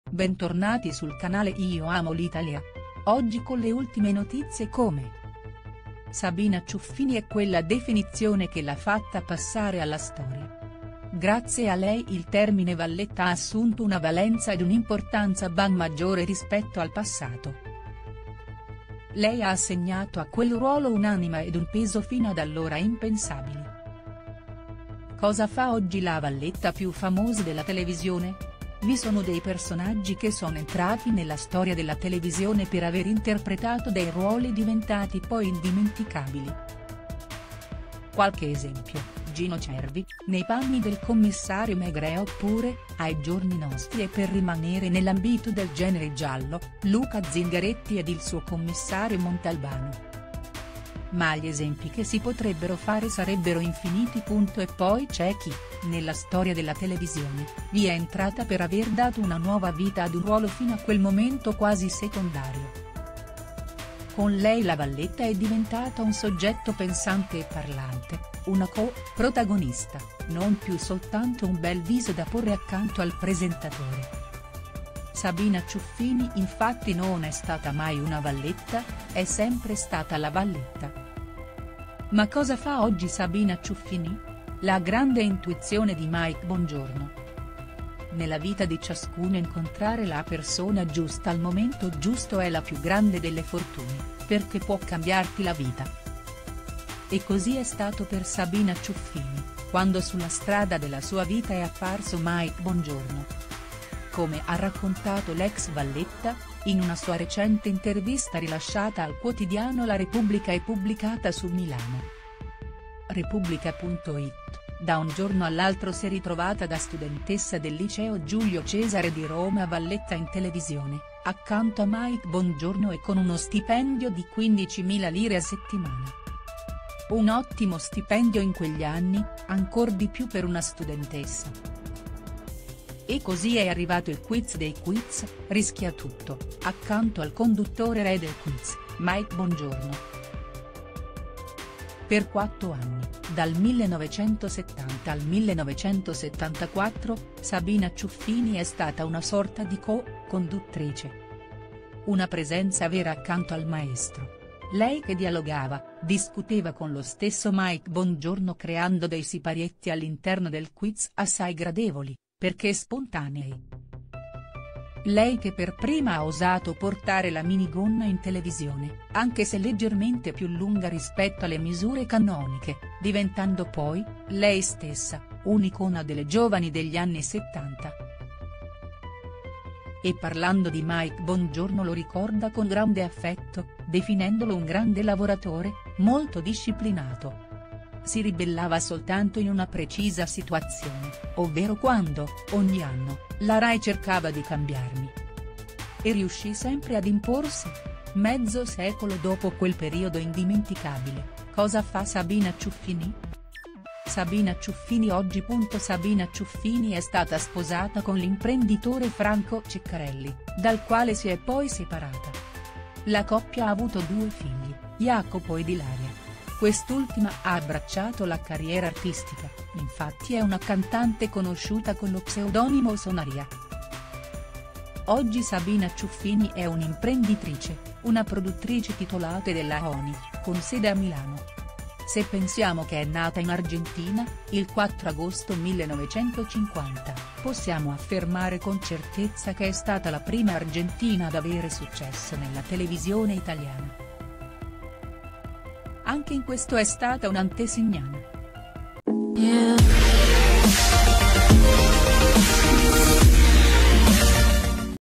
Ricordate Sabrina Ciuffini la prima valletta della tv Oggi è così e non crederete ai vostri occhi
Sabina Ciuffini e quella definizione che l’ha fatta passare alla storia. Grazie a lei il termine “valletta” ha assunto una valenza ed un’importanza ben maggiore rispetto al passato. Lei ha assegnato a quel ruolo un’anima ed un peso fino ad allora impensabili. Cosa fa oggi la valletta più famosa della televisione?

Vi sono dei personaggi che sono entrati nella storia della televisione per aver interpretato dei ruoli diventati poi indimenticabili. Qualche esempio: Gino Cervi, nei panni del Commissario Maigret oppure, ai giorni nostri e per rimanere nell’ambito del genere “giallo”, Luca Zingaretti ed il suo Commissario Montalbano. Ma gli esempi che si potrebbero fare sarebbero infiniti.